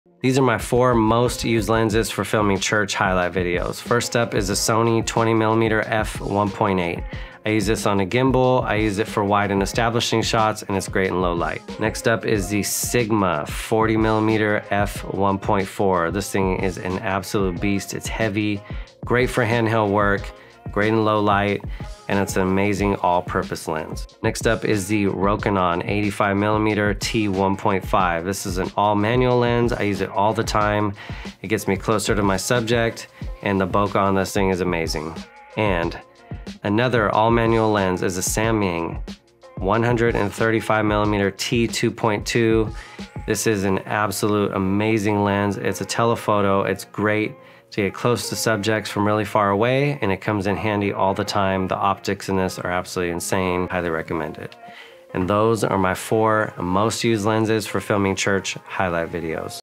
My top 4 Lenses for Filming Church Highlight Videos
These are my 4 go-to lenses for filming church highlight videos. I do use other lenses as well but these cover most of what I need at any given time. One more honorable mention (not talked about in the video) is the Zeiss 55mm f/1.8. I mainly use that one for talking head/interviews when needed.

Sony 20mm f/1.8
Sigma 40mm f/1.4
Rokinon 85mm EF version t/1.5
The Samyang version is no longer available so here's the Rokinon version
You may be able to find the Samyang version on ebay though.